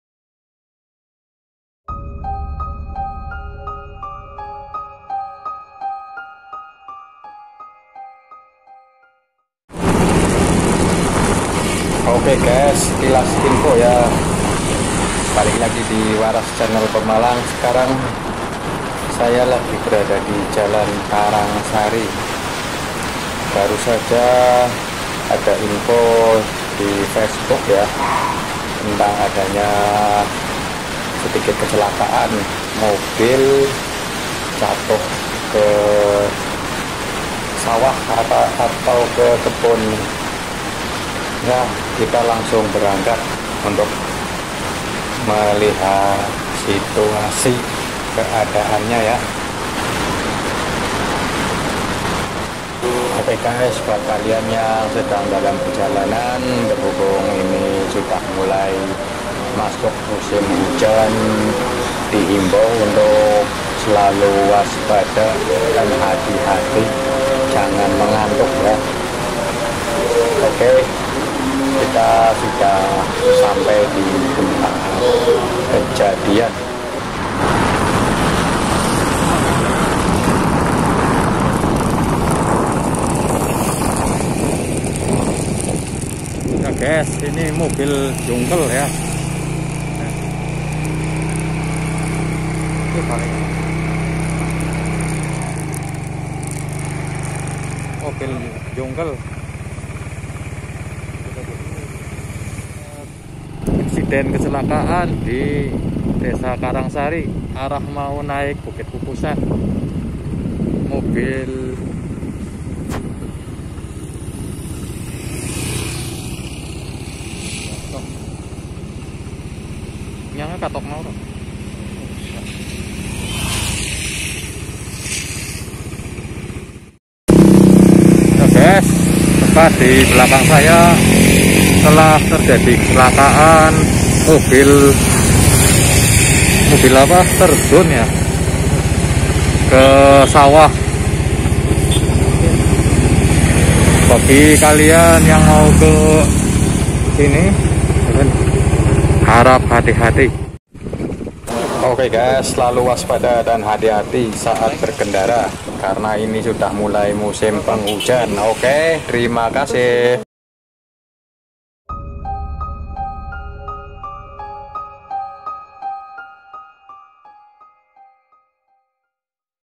MOBIL JATUH TERPEROSOK KE PARIT DI DESA KARANGSARI - PEMALANG
Sebuah insiden Sebuah mobil pengangkut ayam jatuh terperosok di Persawahan Warga..